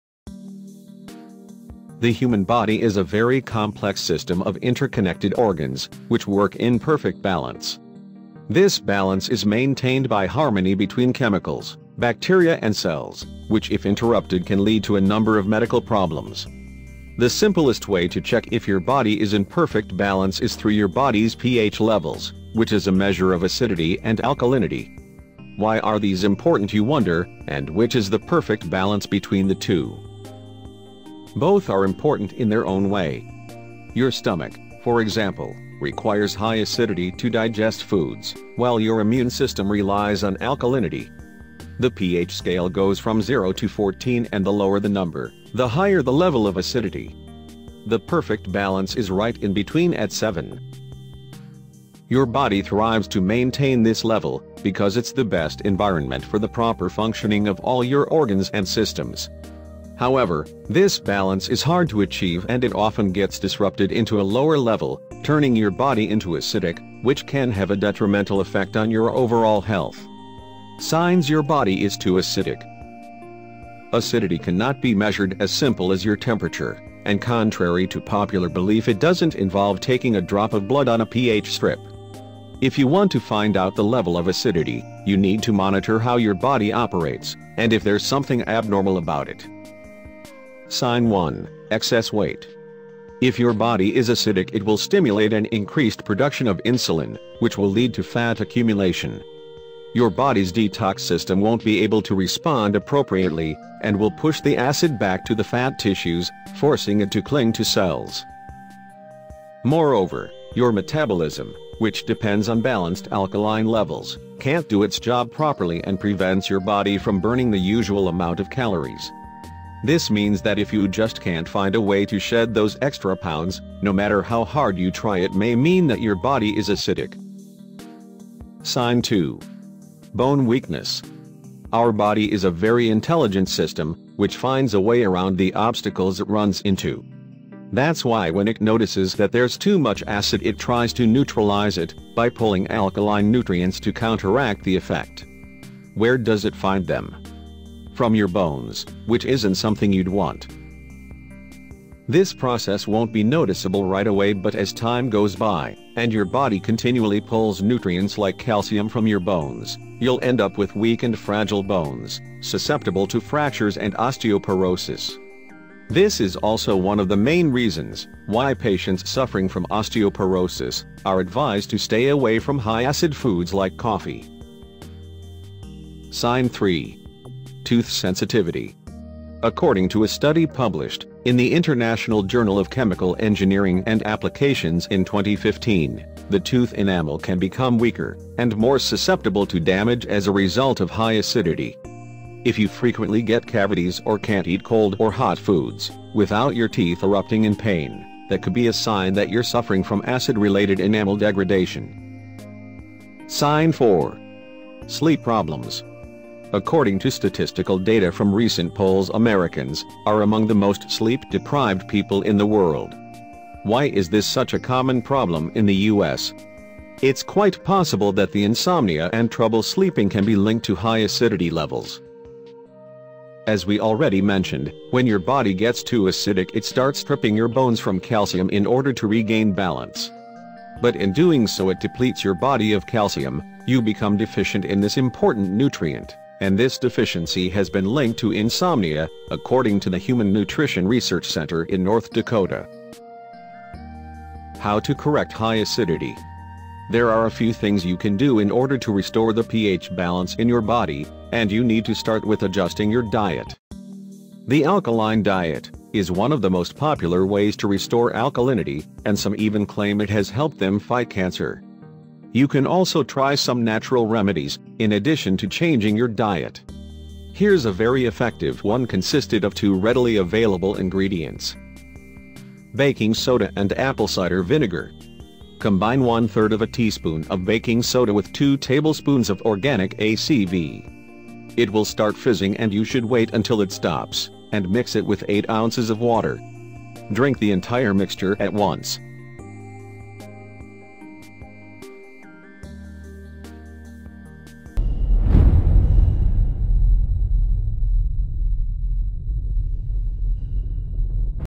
4 SIGNS YOUR BODY IS TOO ACIDIC AND HOW TO FIX IT
If you want to restore the alkalinity you need to eat lots of:

Kale
Broccoli
Cucumber
Spinach
Arugula
Collards
Zucchini
Soybeans
Tofu
Nuts
Seeds
Legumes
Raisins
And eat less or even eliminate entirely from your diet:

Dairy
Eggs
Meat
Most grains
Packaged, processed snacks
Canned foods
Caffeine